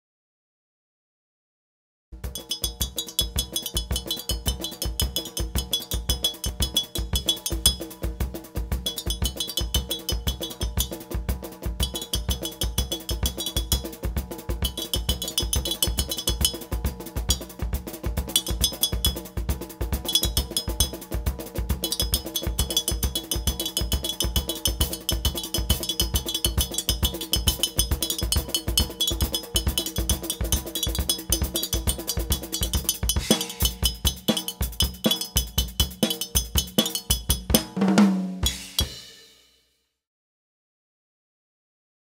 Frigideira live "Stefano Rossini Batuque Percussion"
Concerto live 2011 solo di percussioni - Frigideira -
"Stefano Rossini Batuque Percussion"